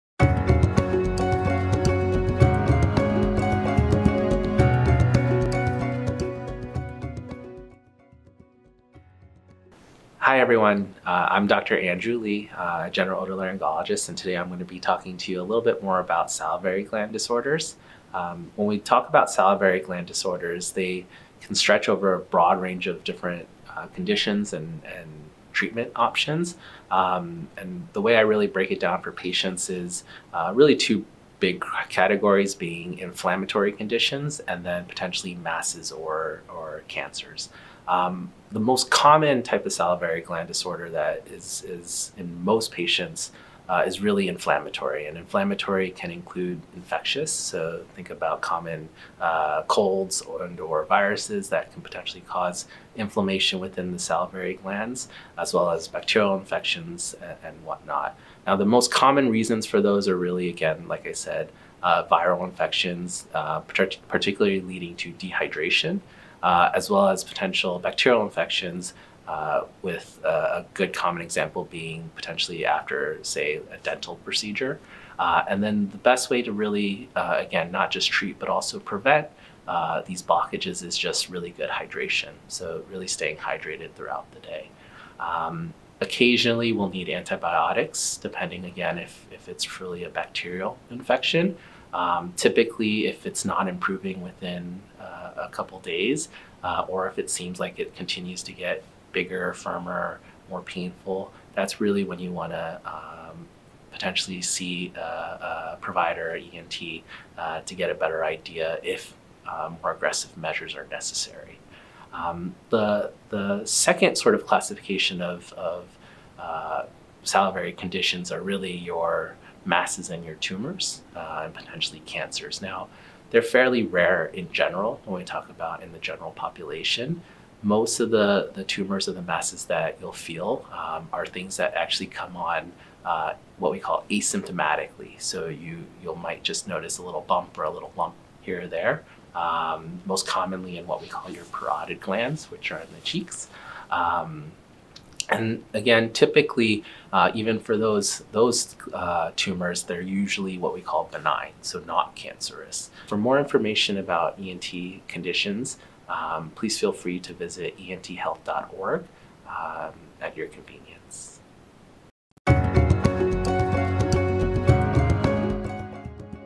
Salivary Gland Disorders
Dr. Andrew Lee describes Salivary Gland Disorders.